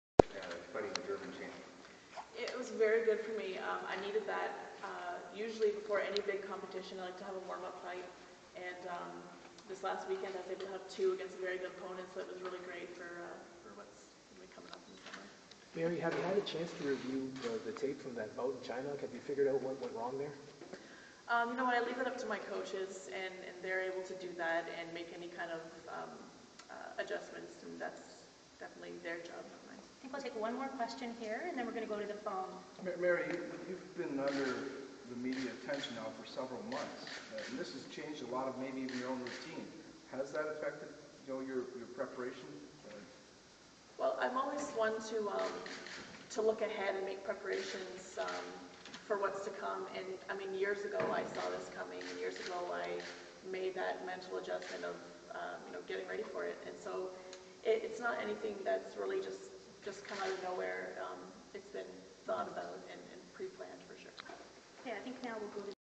Mary Spencer Olympic announcement
Windsor boxer Mary Spencer talks about earning  wildcard for the London Games.